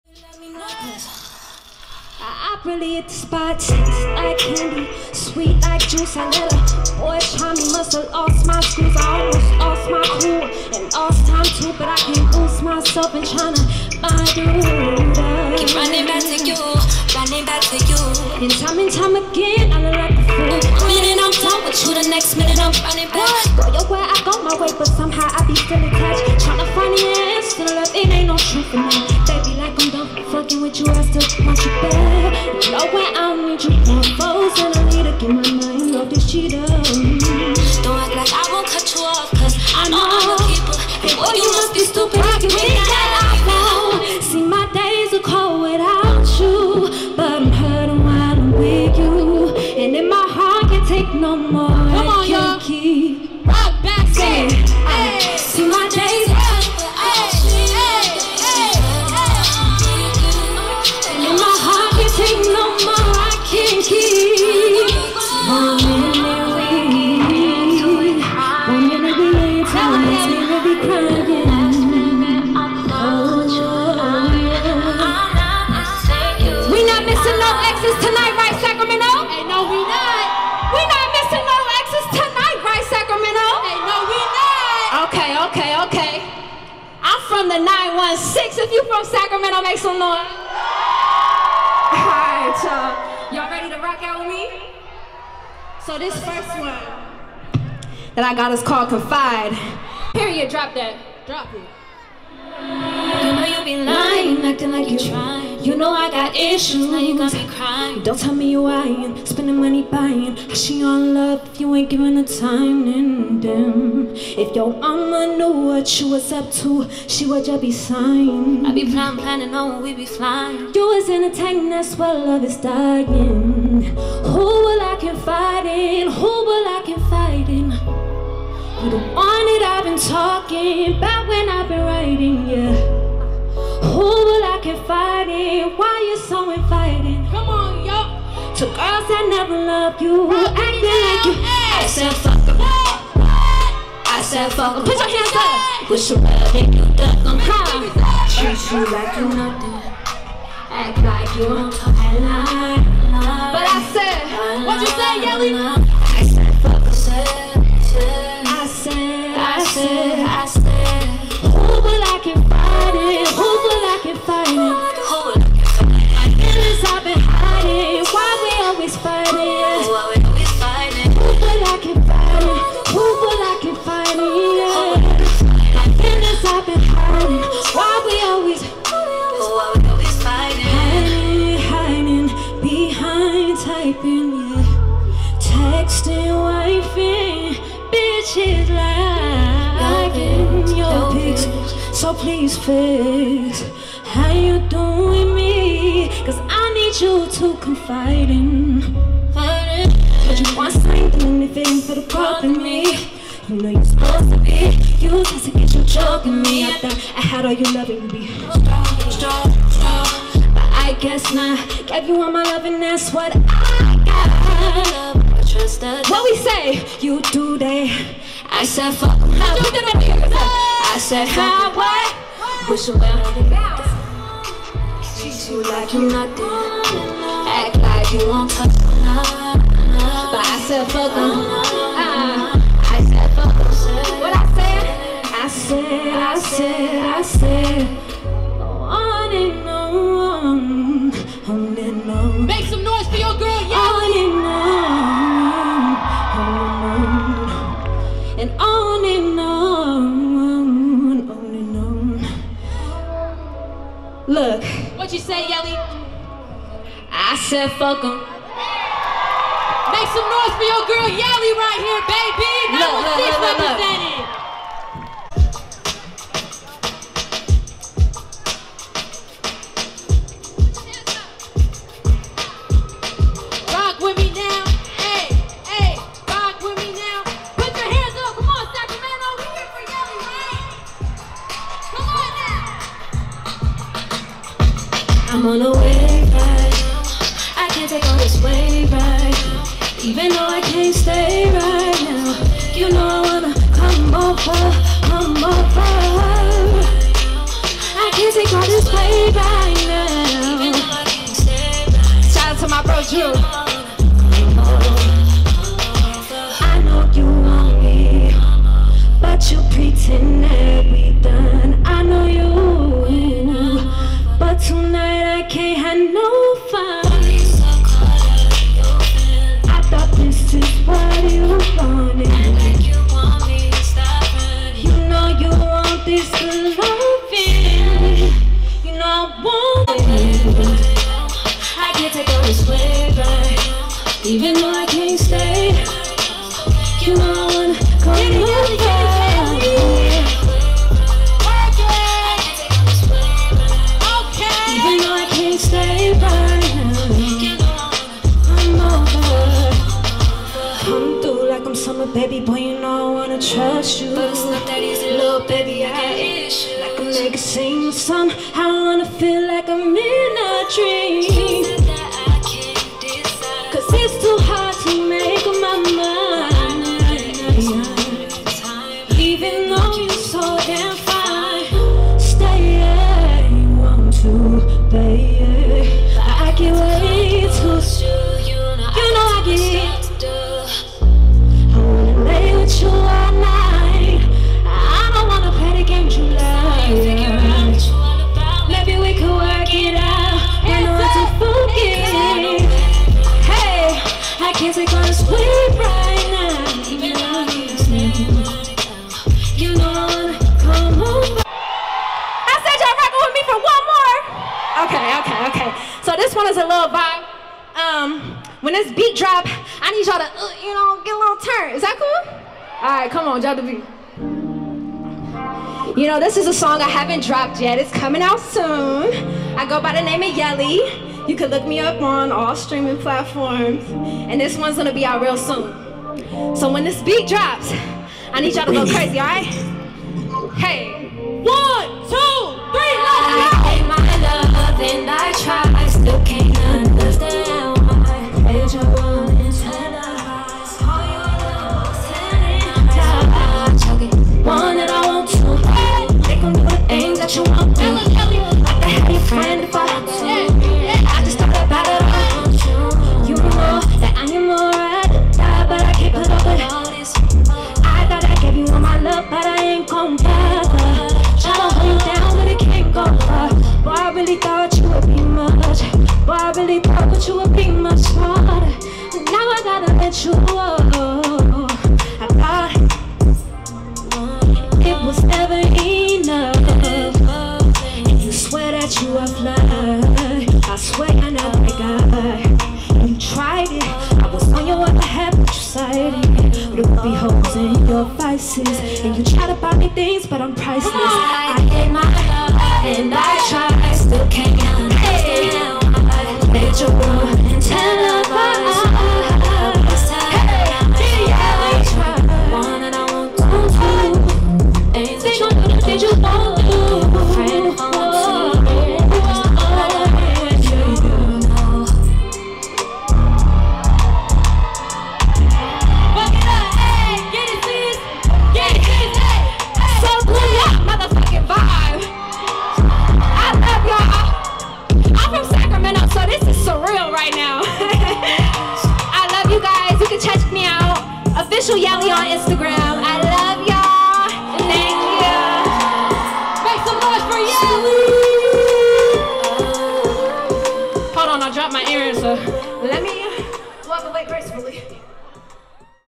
YELLY (SACRAMENTO's OWN) | LIVE PERFORMANCE @ SOL BLUME 2022 | Discovery Park, Sacramento, CA
Filmed by Sol Blume 2022 Livestream Equipment
Official Yelly | Day 1, April 30, 2022